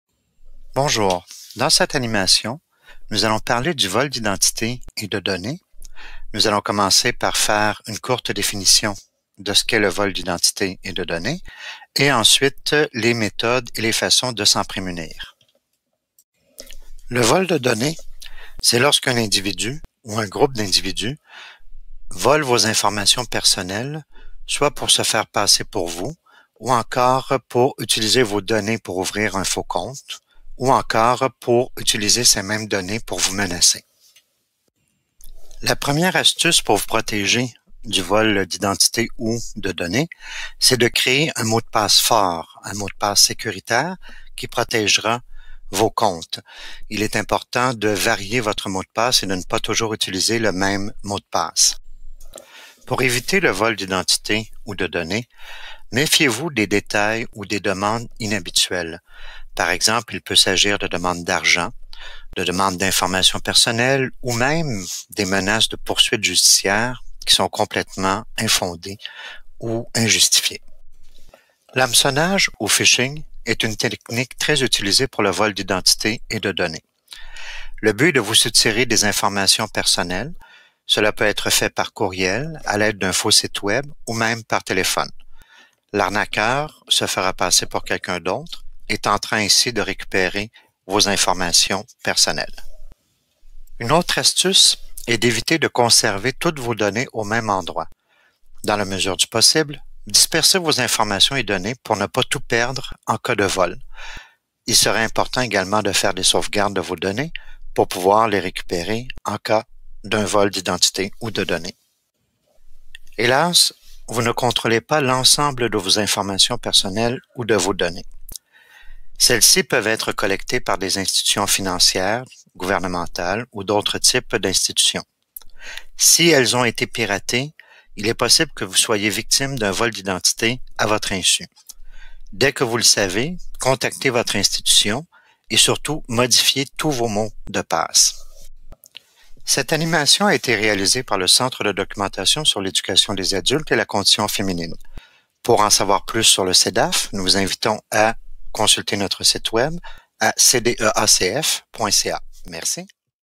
Vol d'identité et de données
Dans cette animation, nous allons parler du vol d'identité.
Nous allons commencer par faire une courte définition de ce qu'est le vol d'identité et de données et ensuite les méthodes et les façons de s'en prémunir.

Retrouvez cette capsule ainsi que d'autres outils de sensibilisation dans la boîte à outils Cybersécurité dans le communautaire disponible sur le site du CDÉACF

0:00 Introduction
0:16 Définition du vol de données
0:35 Avoir un mot de passe fort et sécuritaire
0:52 Se méfier des détails inhabituels
1:11 Qu'est-ce que le hameçonnage?
1:33 Éviter de conserver toutes vos données au même endroit
1:52 Quoi faire si votre institution a été piratée
2:19 Crédits